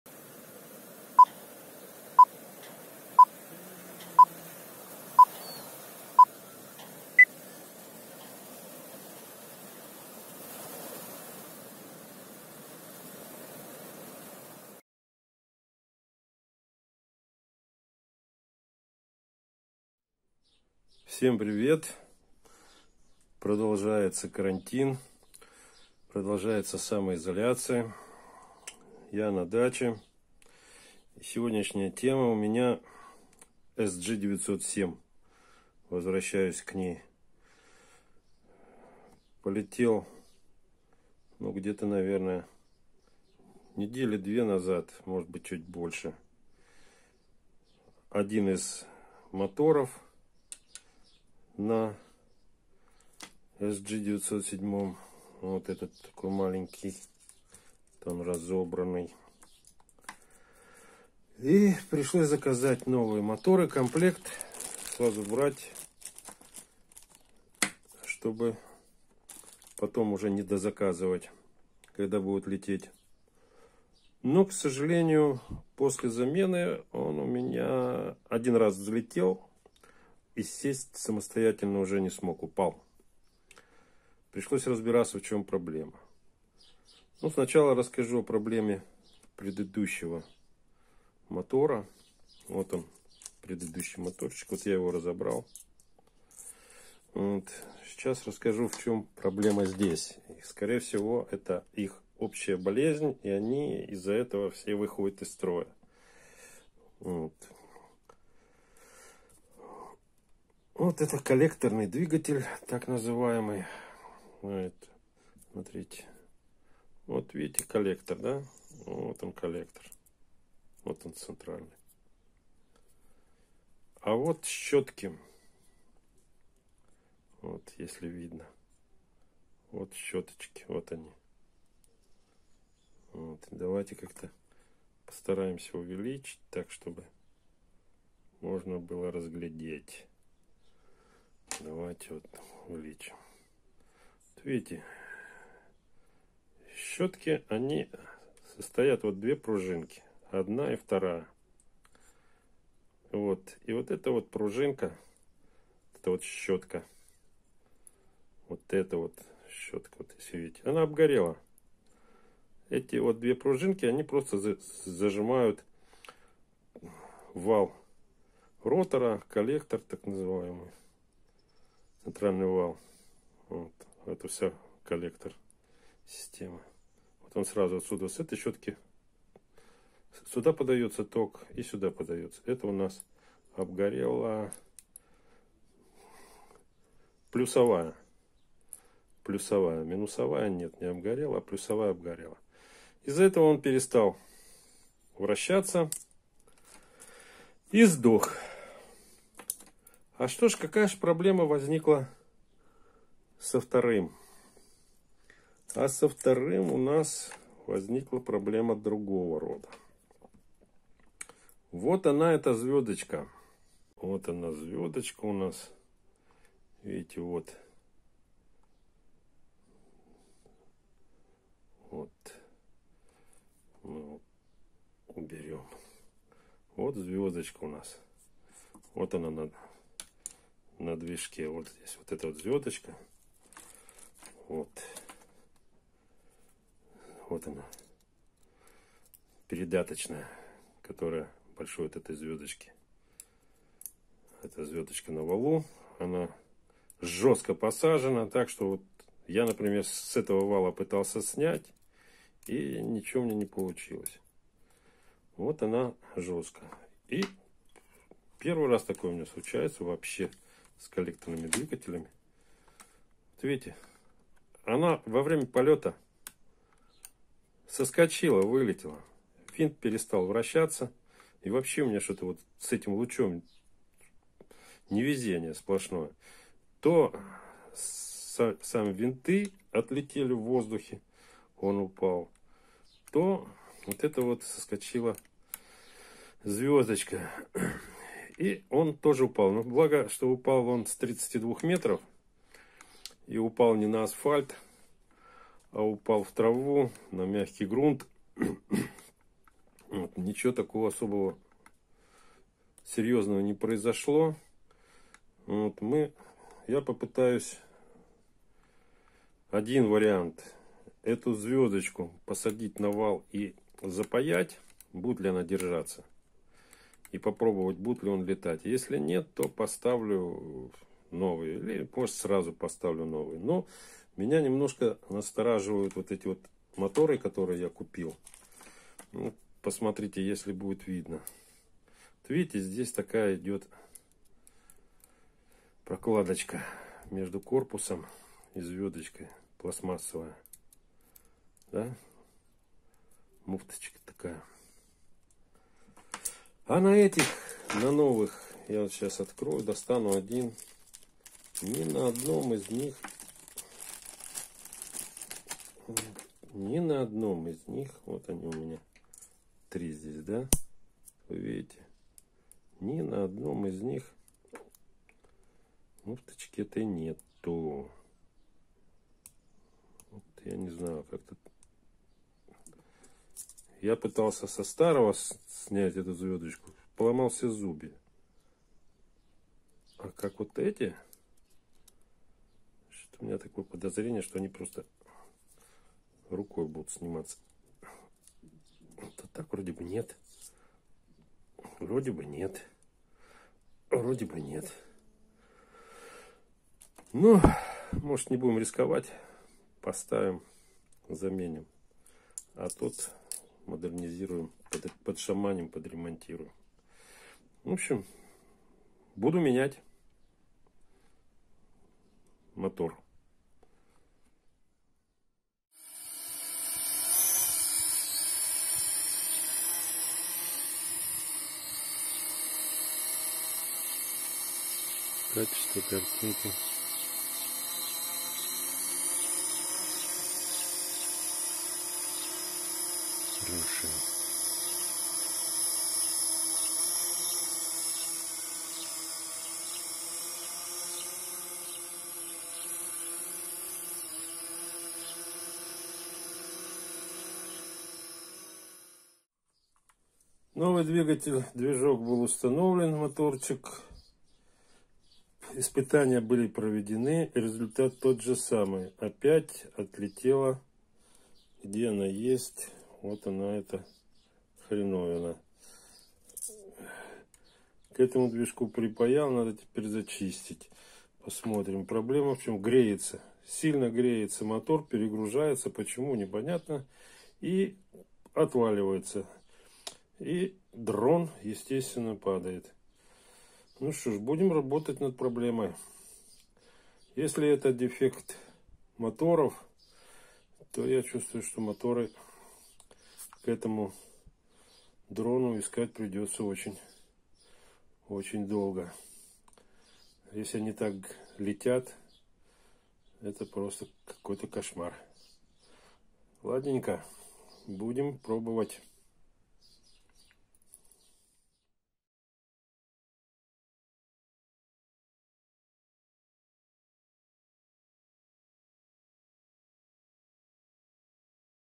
SG907 - проблема с моторами.
SG907 - проблема с моторами. У всего есть свой срок службы. И коллекторные моторы не исключение. На SG907 они выходят из строя чаще, чем на подобных моделях. А при замене возникают проблемы. С такими проблемами столкнулся и я. Думаю, что это последнее видео из серии "SG907".